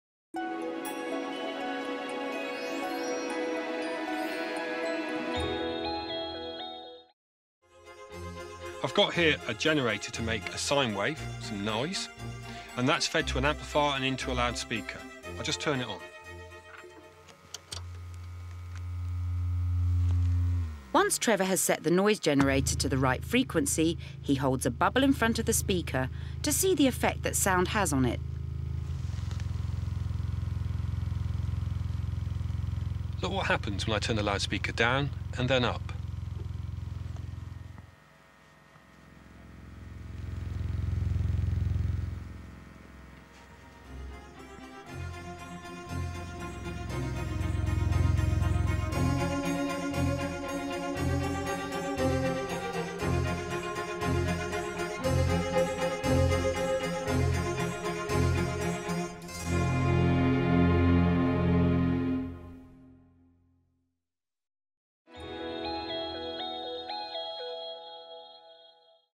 Teachers TV: Sound Through a Medium: Wobbling Bubbles
This quick practical experiment showing how a bubble is affected by different sounds is designed for use as a Year 7 to 11 physics classroom resource.

This Key Stage 3/4 science test shows how a bubble vibrates at different rates when placed near a loudspeaker, which changes the sound frequency. Licensed to CPD College Ltd.